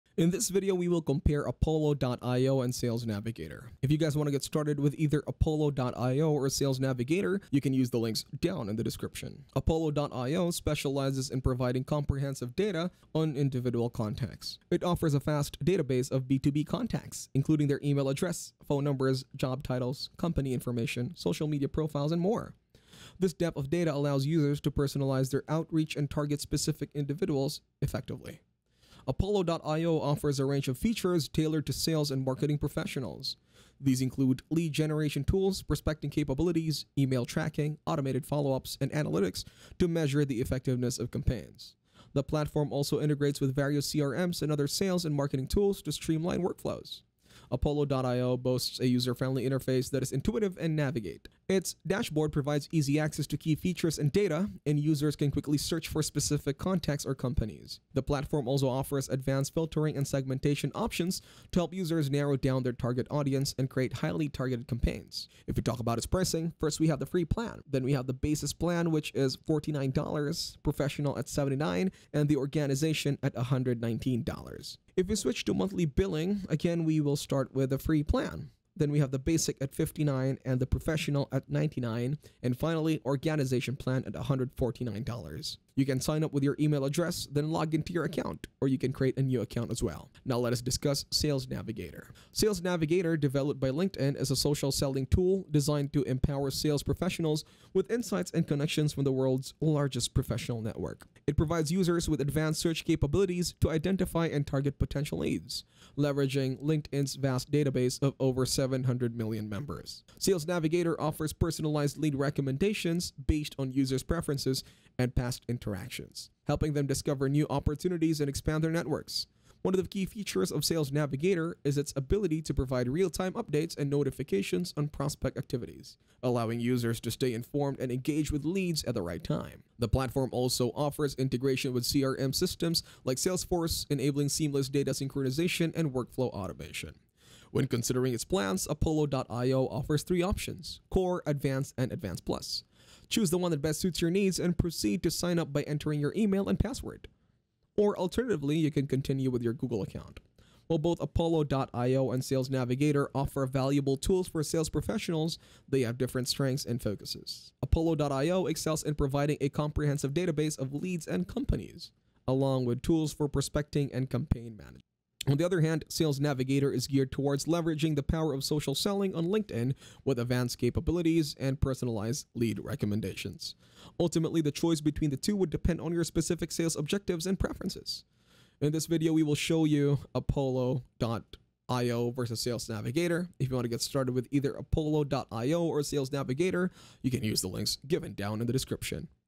APOLLO IO VS SALES NAVIGATOR (2026) | Which is Better?
In this video I'll show you comparison betweem apollo io vs sales navigator